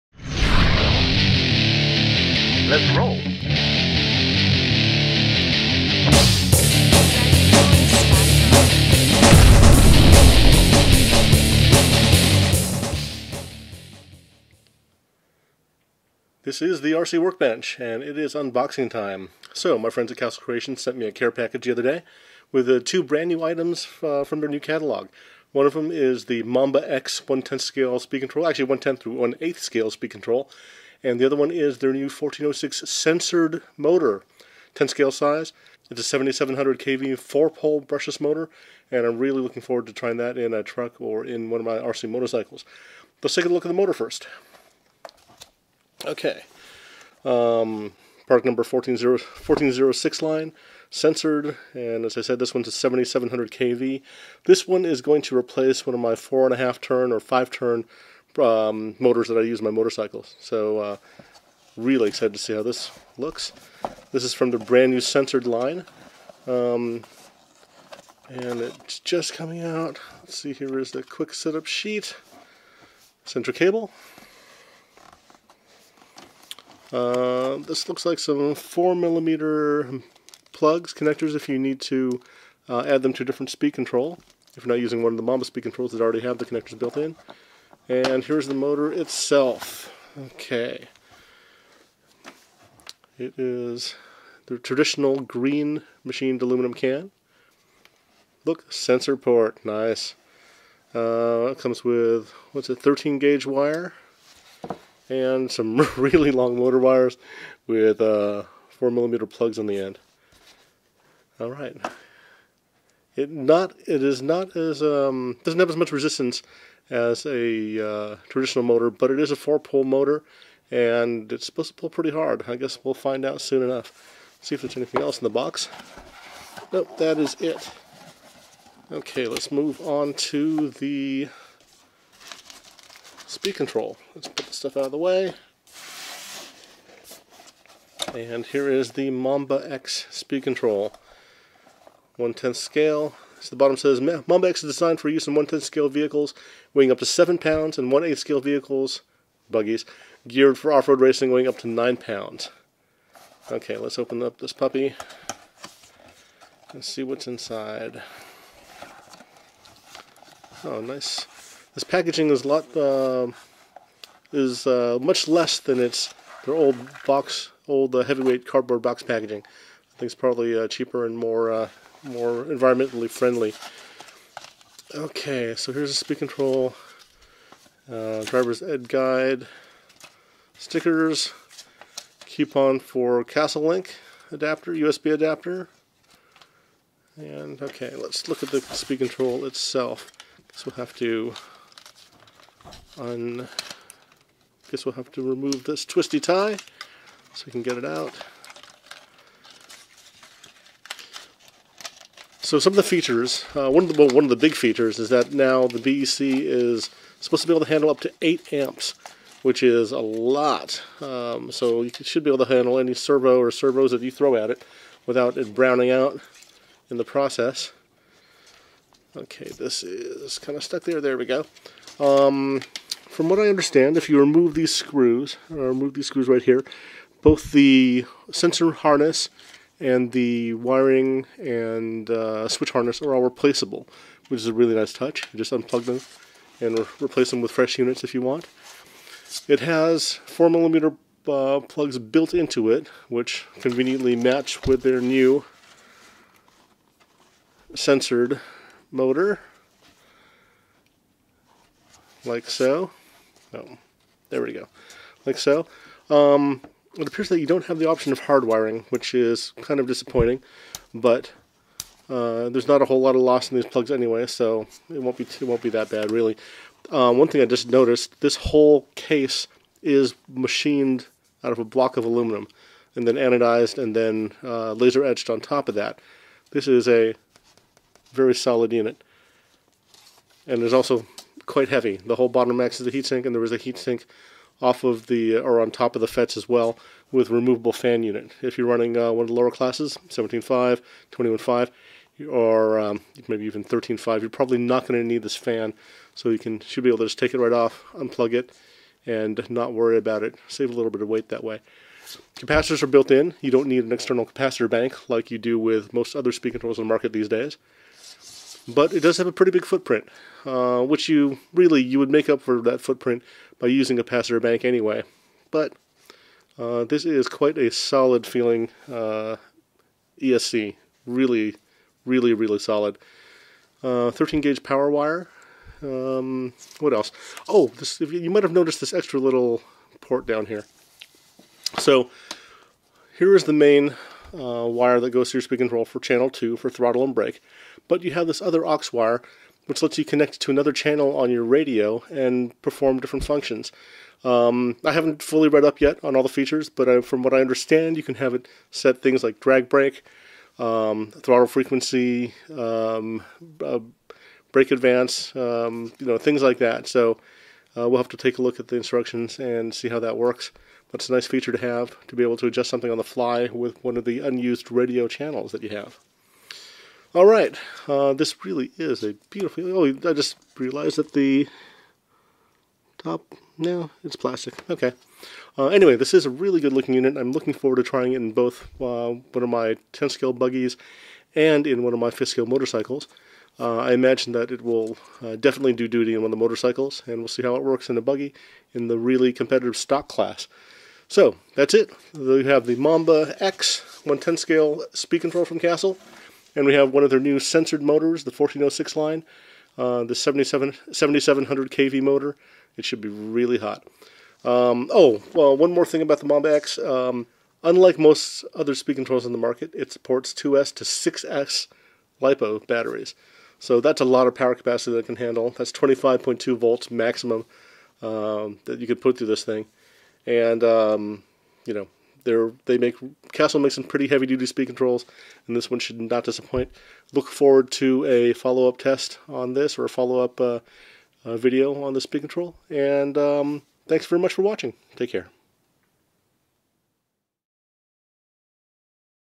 Castle Creations Mamba X and 1406-series Sensored Motor Unboxing
This is a quick unboxing of Castle Creations' latest brushless motor and ESC, the sensored (that's right – a sensored motor from Castle!!!) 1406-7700KV motor and their newest 2S-6S ESC, the Mamba X.